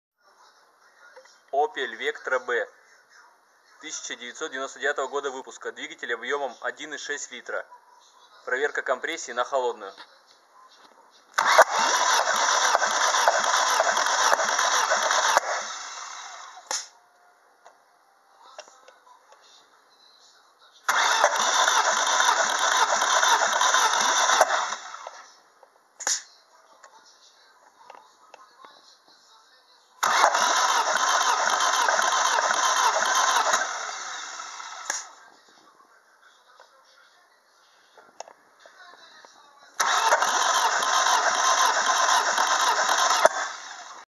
Двигатель Opel для Vectra B 1999-2002
Описание двигателя Двигатель Opel для Vectra B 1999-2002
1.6Л. 16V КОМПРЕССИЯ: 1Ц:12 2Ц:12 3Ц:12 4Ц:12 X16XEL  (СОПРОВОДИТЕЛЬНЫЙ ПАКЕТ ДОКУМЕНТОВ:  ГТД )  ПО ЗАПРОСУ ПОКАЖЕМ СОСТОЯНИЕ ЗАПЧАСТИ ПО ВИДЕО 601643